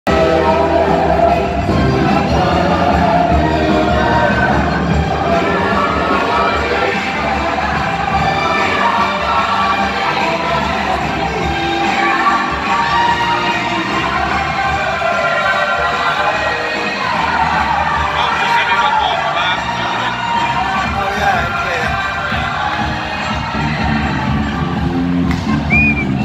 HB Barnum
Such a gentleman he's filming on the dance floor and I'm filming HB LOL  at the Soul Festival 4 Winter gardens Blackpool